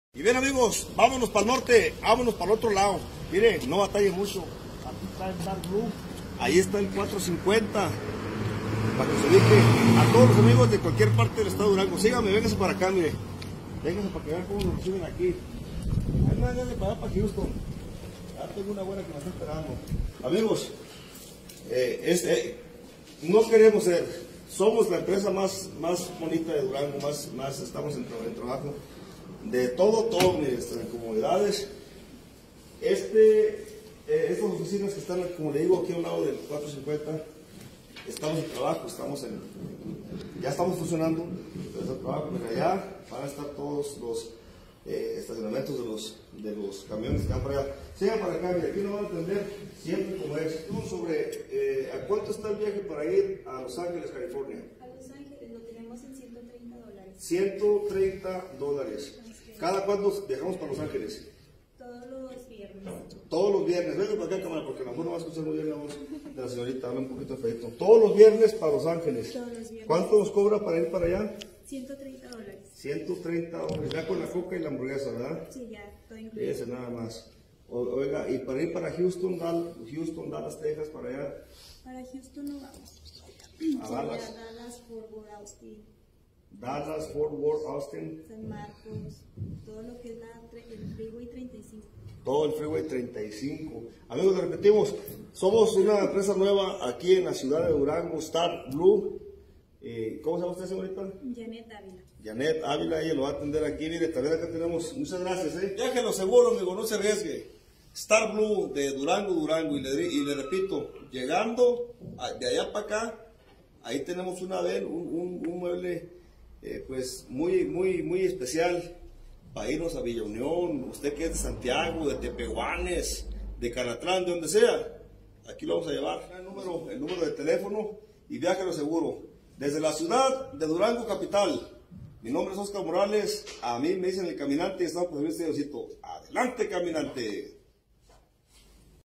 🚌😉Conoce la empresa Starblue con El Caminante de Luis Moya Durango
Viaja a Chicago Il, Dallas Tx, Oklahoma, Wichita Ks, Las Vegas Nv, Los Ángeles, San José Ca. Phoenix y muchos más. Desde Durango, Zacatecas y Calvillo Aguascalientes.